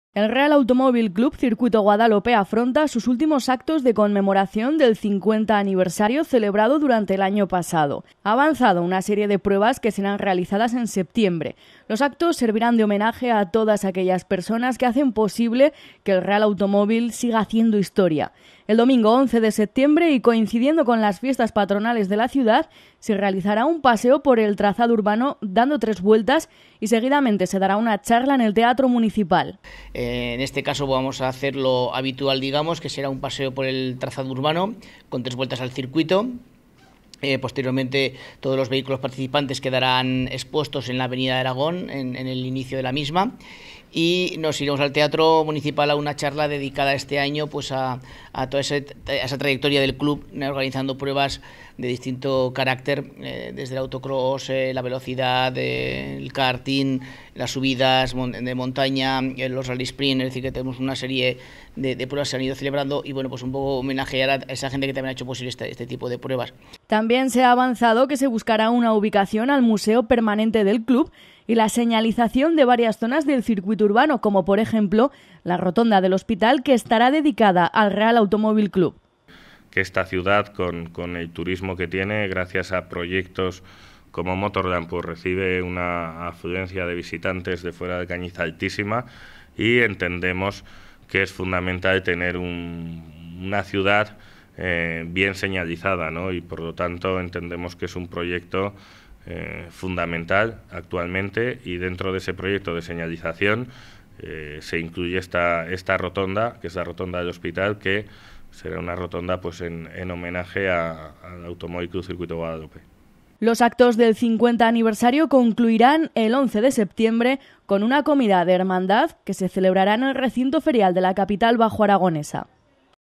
La Comarca.tv - Últimos actos del 50 aniversario del R. A. C. C. Guadalope
Los actos del 50 aniversario del Real Automóvil Club terminarán el próximo 11 de septiembre con un paseo de tres vueltas al trazado urbano, una charla en el teatro y una comida de hermandad en el recinto ferial. Se presentaron los actos en rueda de prensa y el presidente del Real Club aprovechó para reivindicar al alcalde las señalizaciones pendientes en el circuito así como el futuro museo.